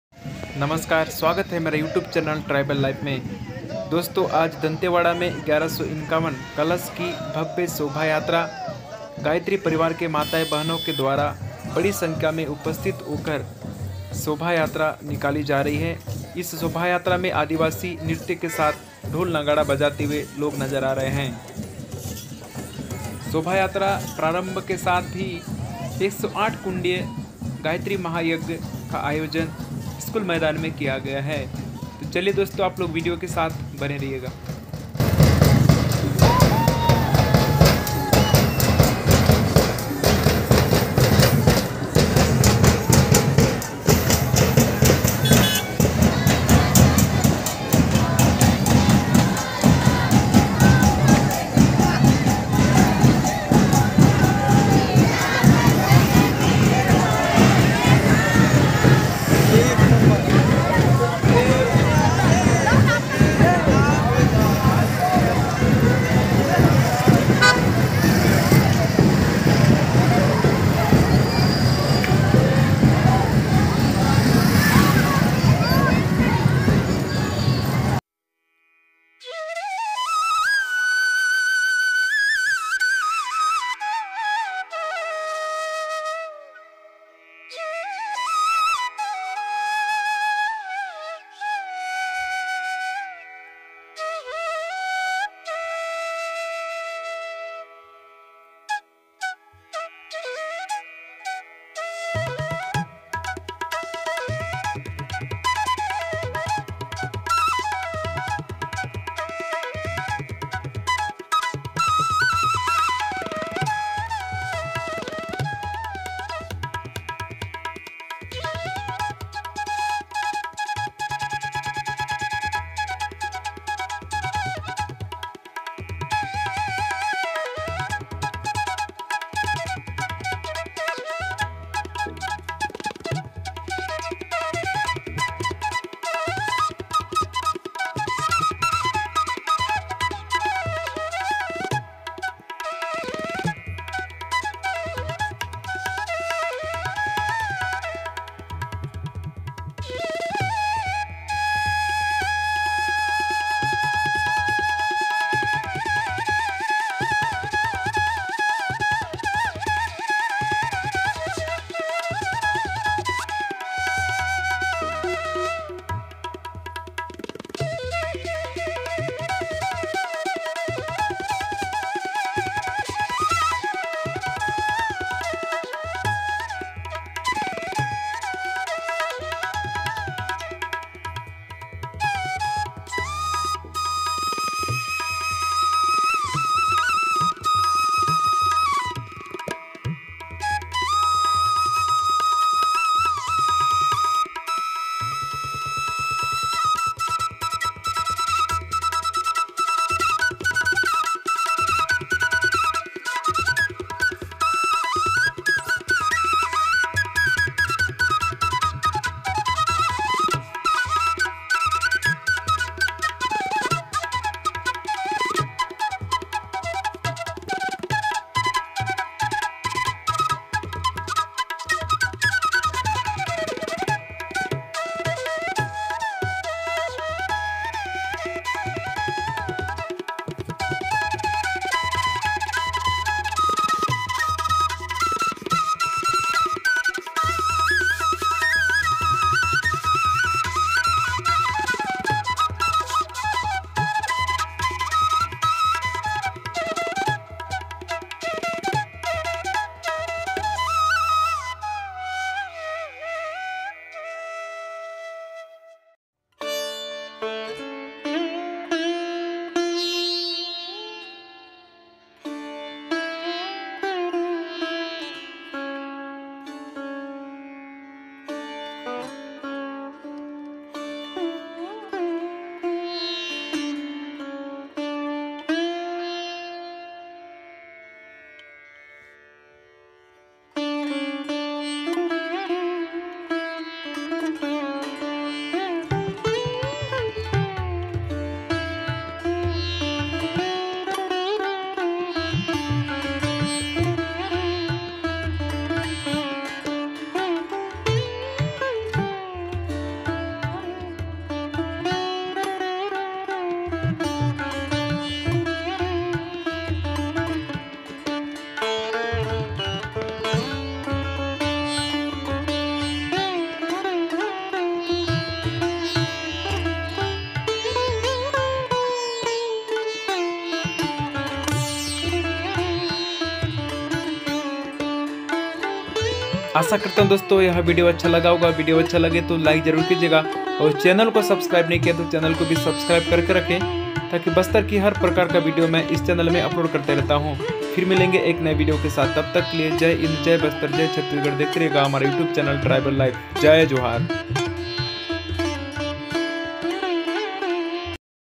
‼️दंतेवाड़ा में गायत्री परिवार के द्वारा 1151 कलश की भव्य शोभायात्रा निकाली गई E TRIBAL LIFE
‼️दंतेवाड़ा में गायत्री परिवार के द्वारा 1151 कलश की भव्य शोभायात्रा निकाली गई E TRIBAL